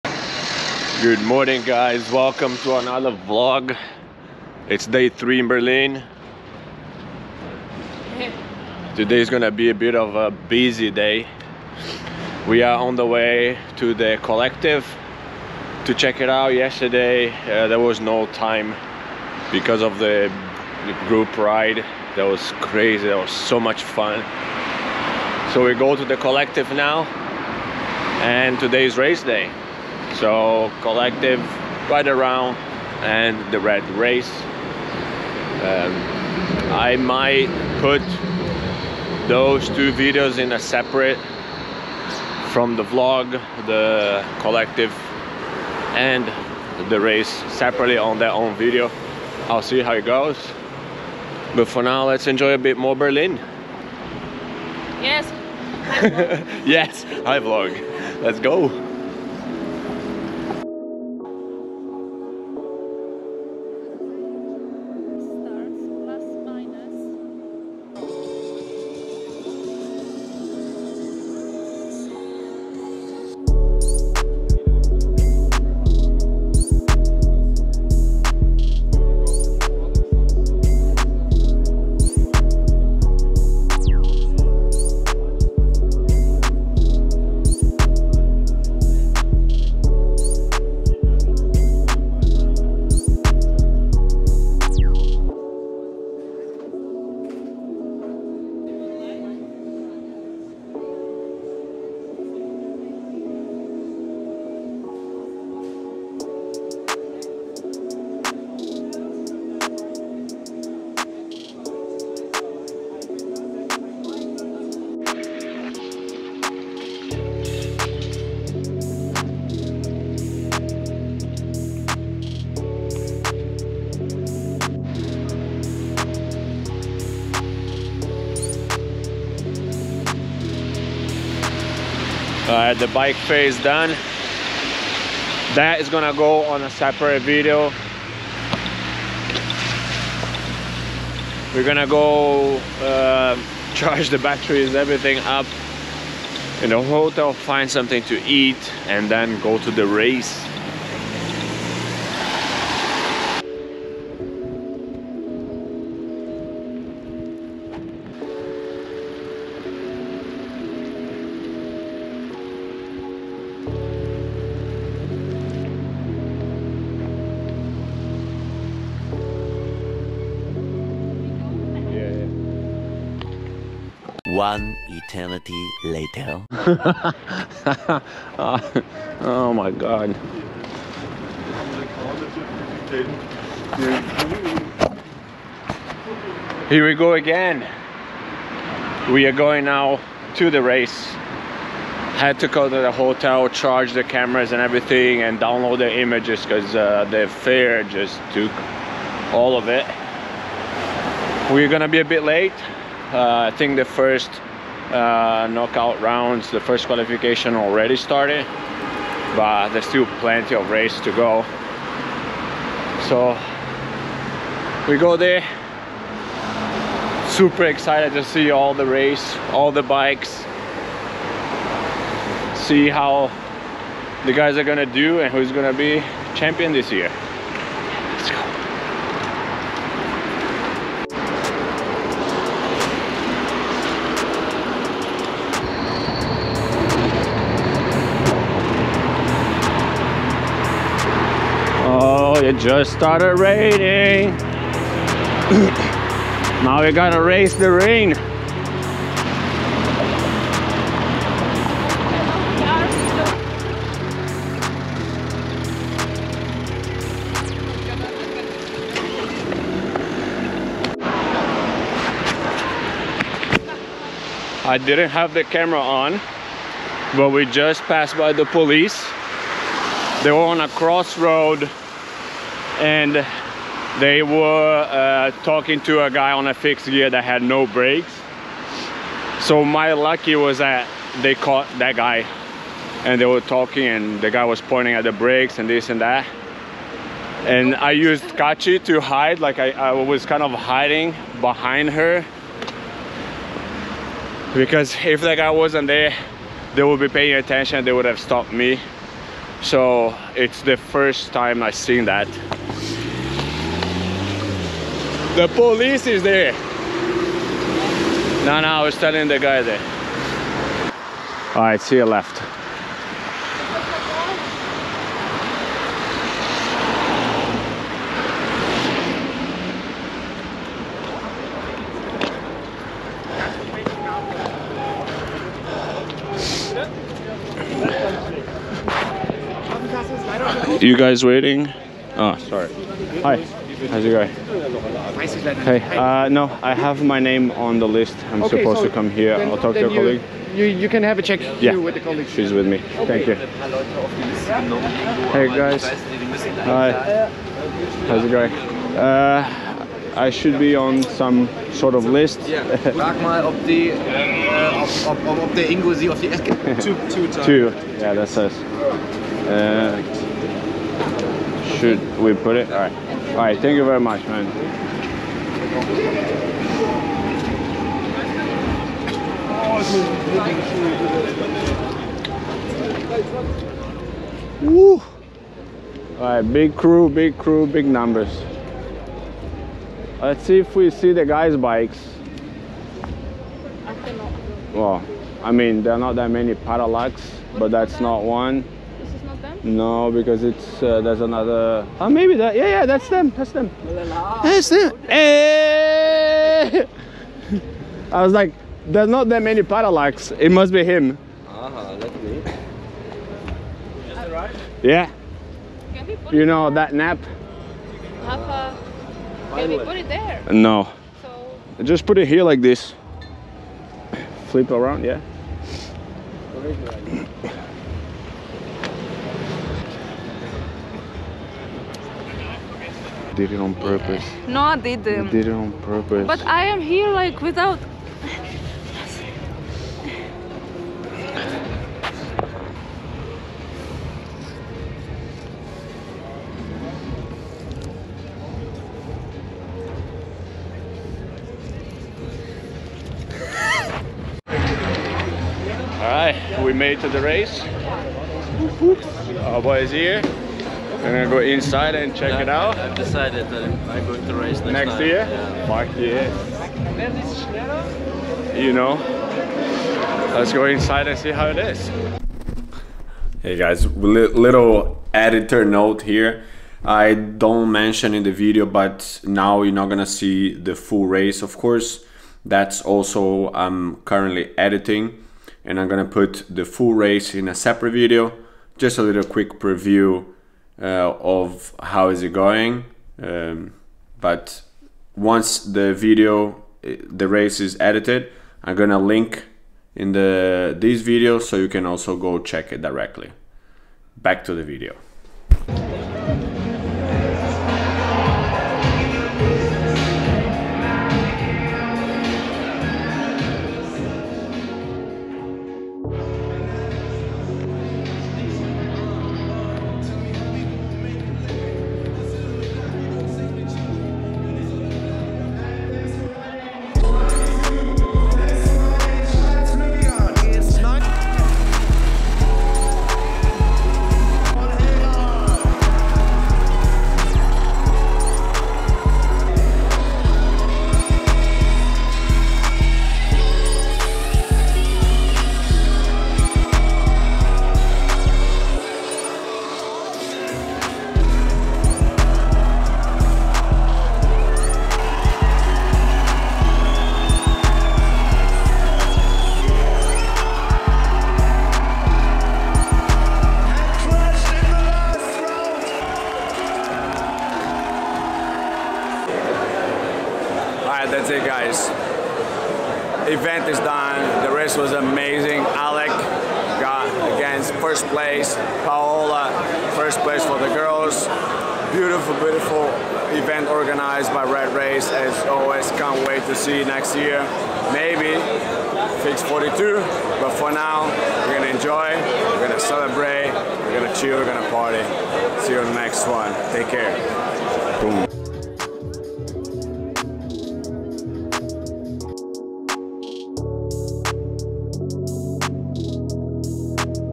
Kolektif fair and RadRace (Day 3) | Random Fixed Gear Vlog
On the last day in Berlin, we managed to visit the Kolektif bike fair and the RadRace!

These are my random vlog series. Just want to bring you with me and see the fixed gear world from my perspective. I hope you enjoy it.